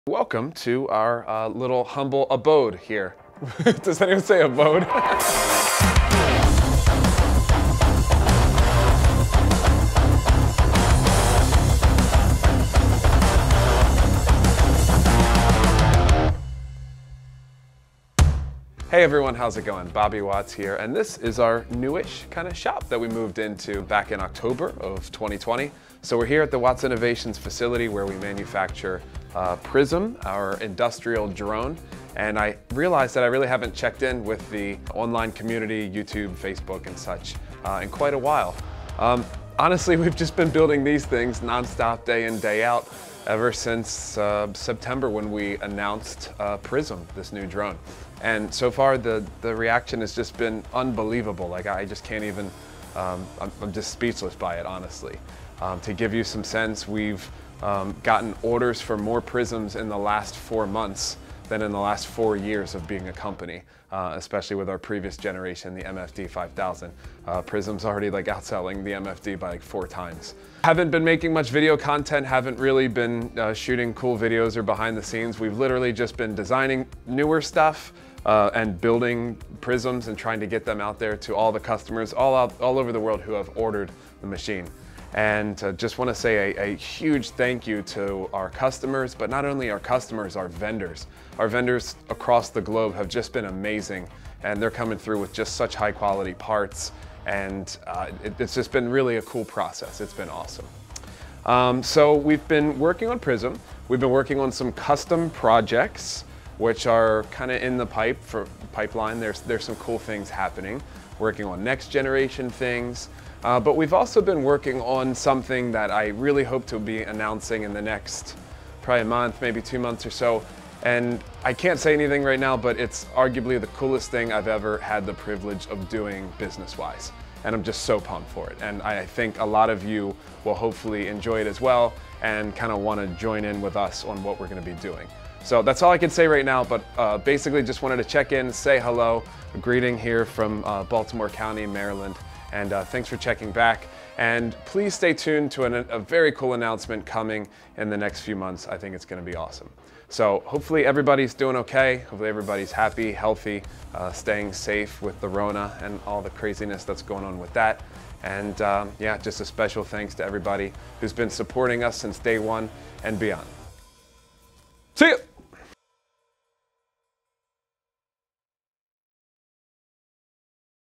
Hello From Watts Innovations! Spring 2021 Update
It's been awhile! The past few months have been incredible, as we have swung into full production of our latest aircraft PRISM. This aircraft was created for the industrial market, appealing to customers who are looking for a drone that is designed, assembled and supported in the USA.

Just wanted to check in with everyone to show what we have been up to these days. Hope you enjoy seeing some behind the scenes clips and hearing more about what's going on here at Watts Innovations HQ.

As mentioned in the video, we have some really exciting things in the works that we look forward to sharing with you all in the coming months.

A special thank you once again to all of our customers and vendors for their support!